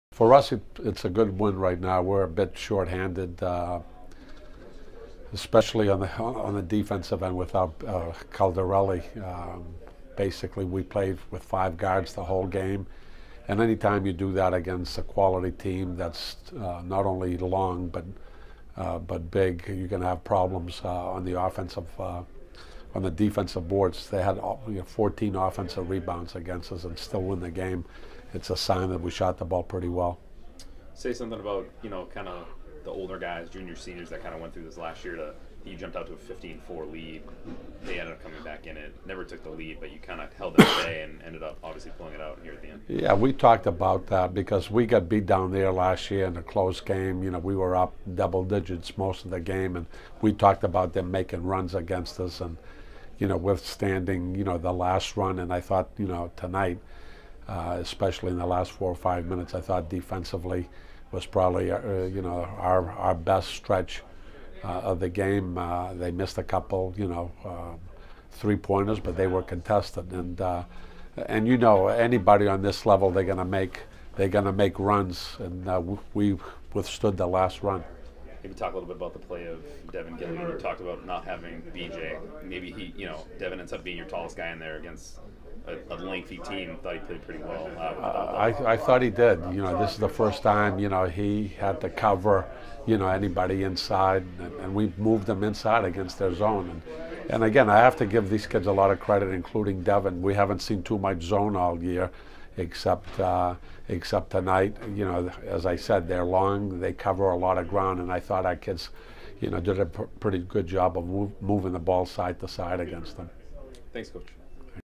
Rapid Reaction: Men's basketball vs. Holy Family
Head men's basketball coach Stan Spirou discusses his team's 74-69 victory over Holy Family on Saturday.